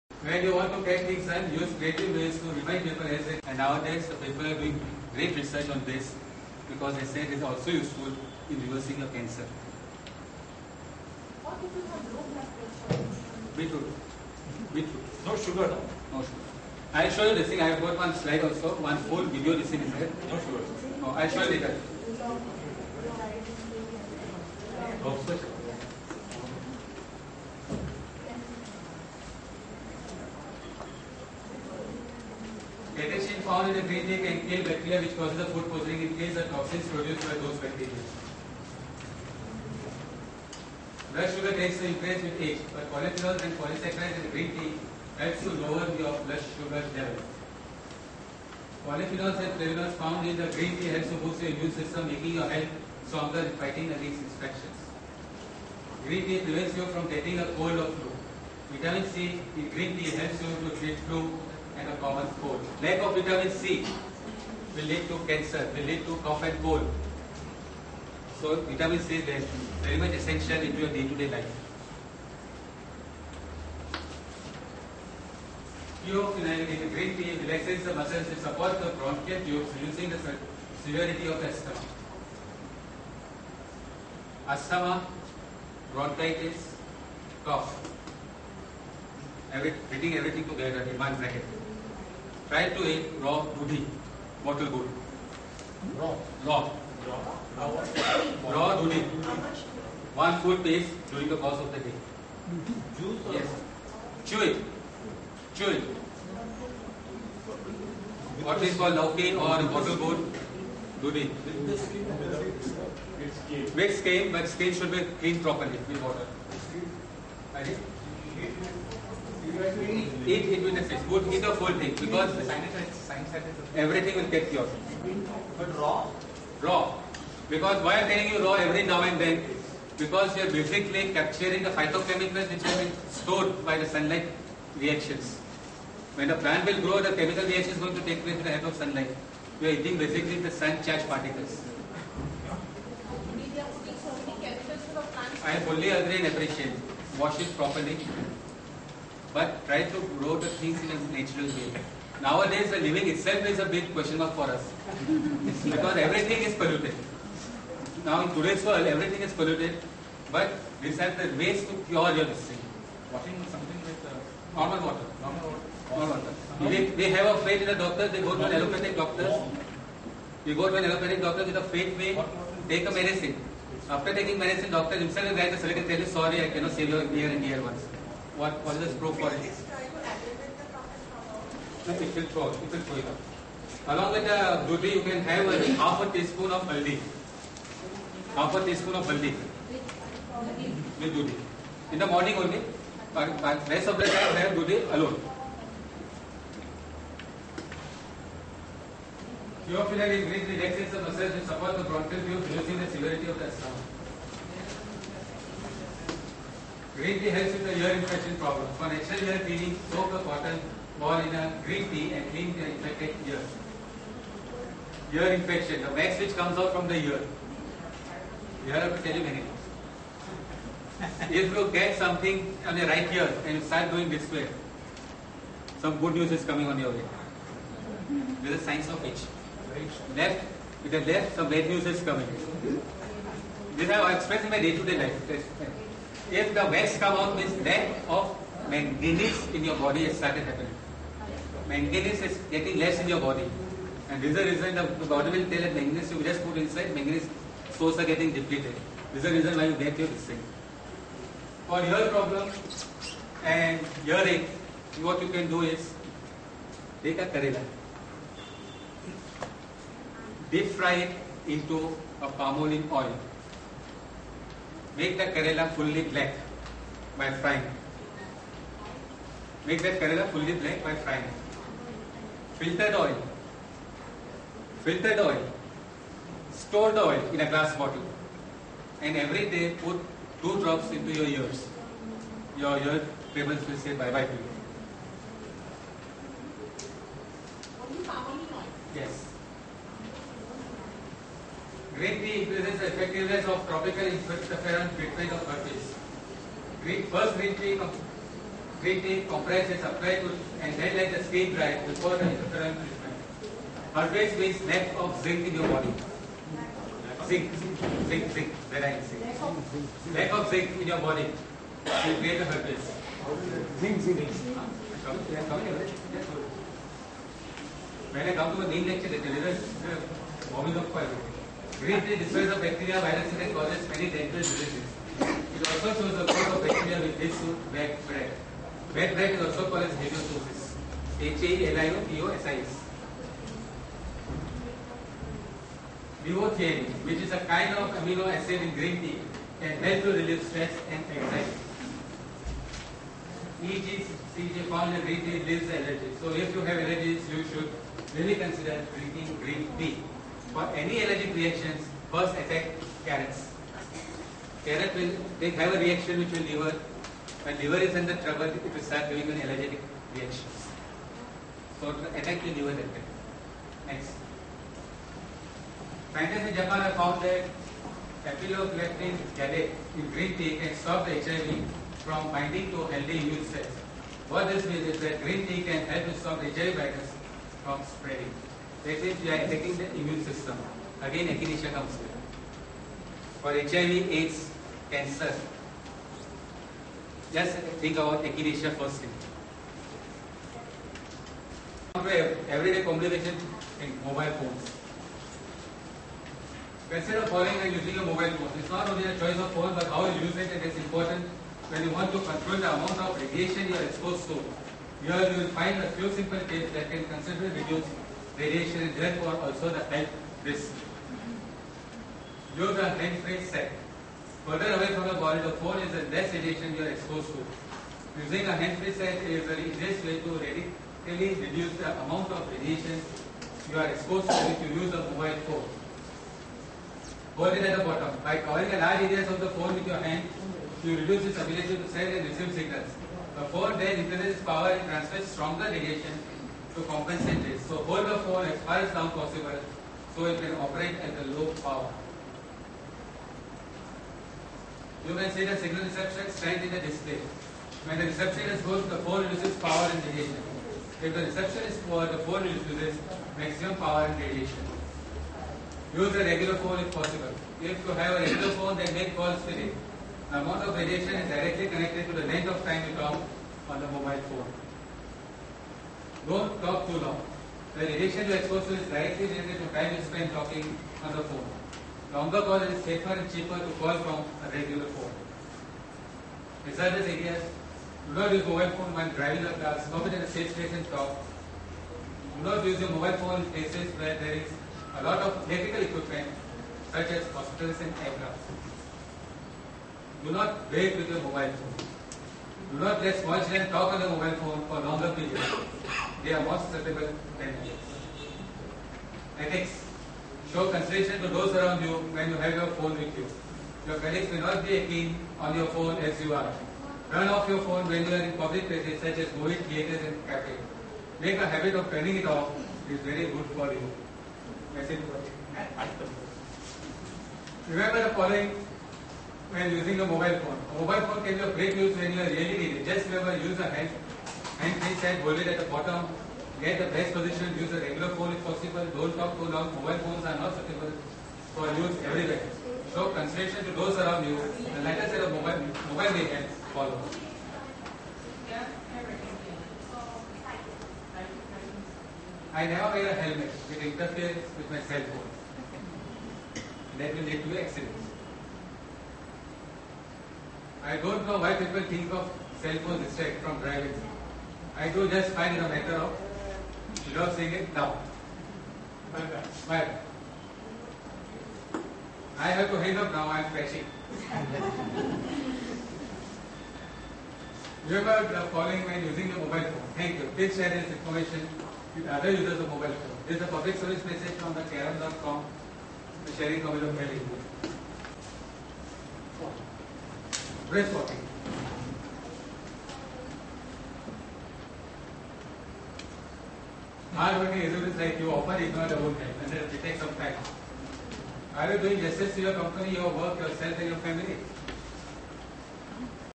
Application Of Home Remedy In Daily Life Part 5
This video is a workshop by Dr.Darius Umrigar at HELP on 24th Oct, 09. Topic "Application of Home remedy in daily Life." Dr.Darius Umrigar can be contacted at . This is part of the HELP Talk series at HELP, Health Education Library for People, the worlds largest free patient education library